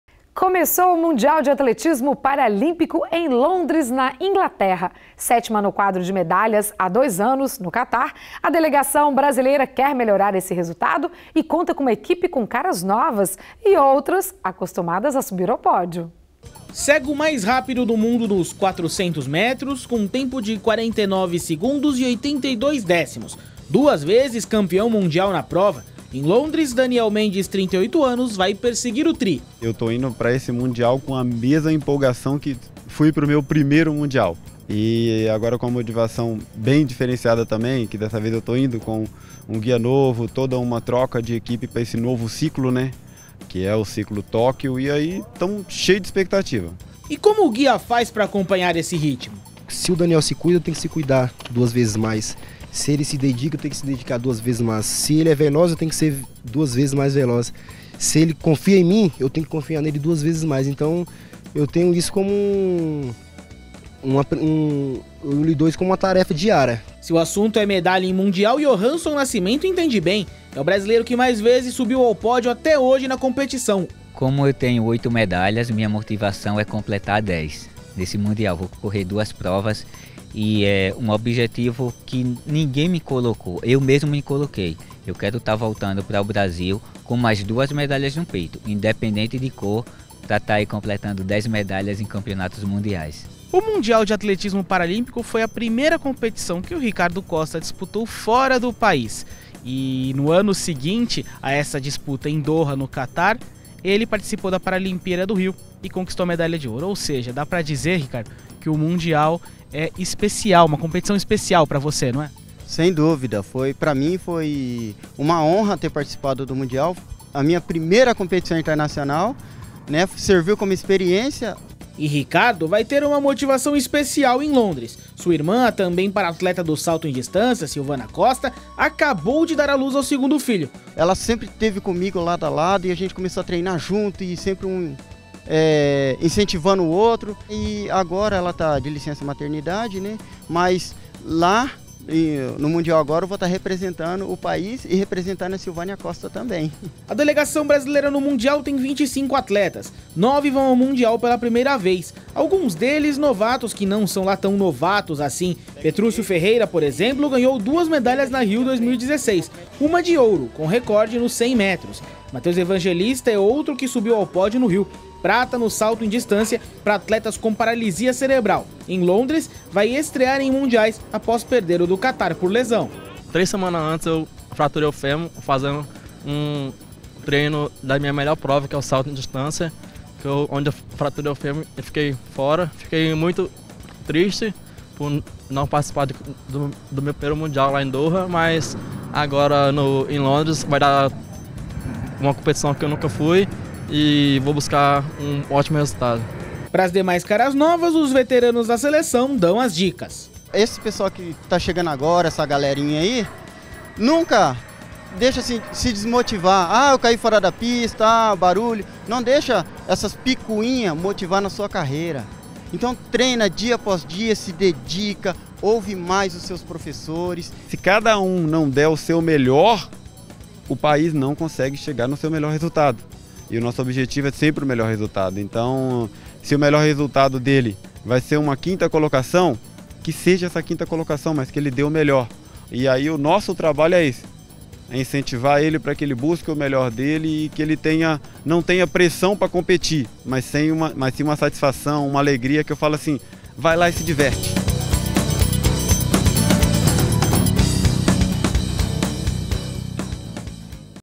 Mundial de Atletismo Paralímpico em Londres
O Mundial de Atletismo Paralímpico de Londres começou na última sexta-feira, mas o atletas bateram um papo com a equipe do Stadium antes de embarcar.